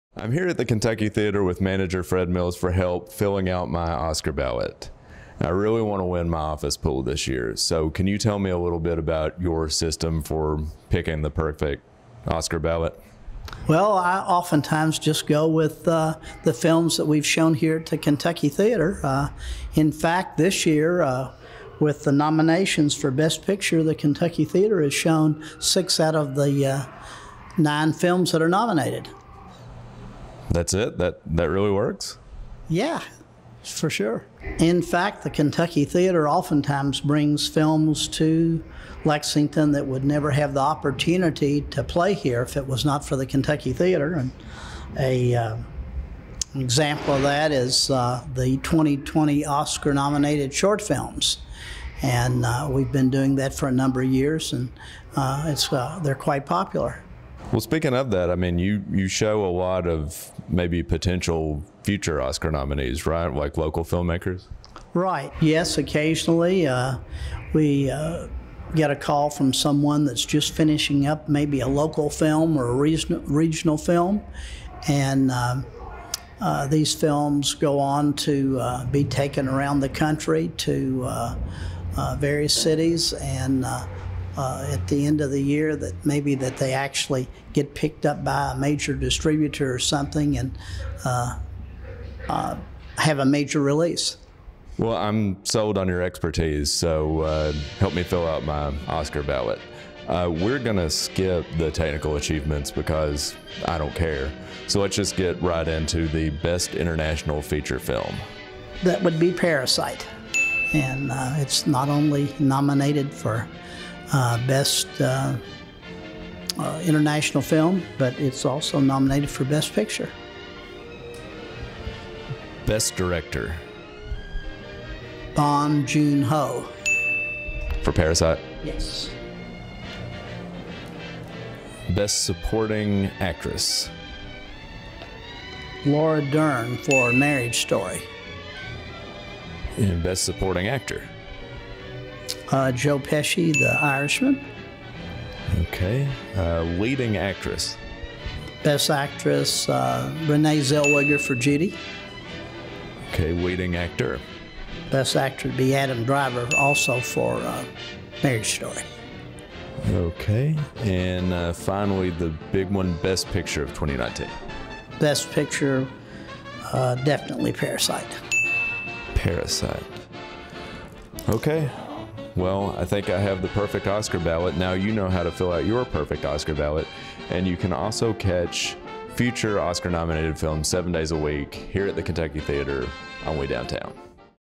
How to Fill Out the Perfect Oscar Ballot with the Kentucky Theater's Fred Mills
Want to complete the perfect Oscar ballot? Kentucky Theater manager Fred Mills shares his winning formula.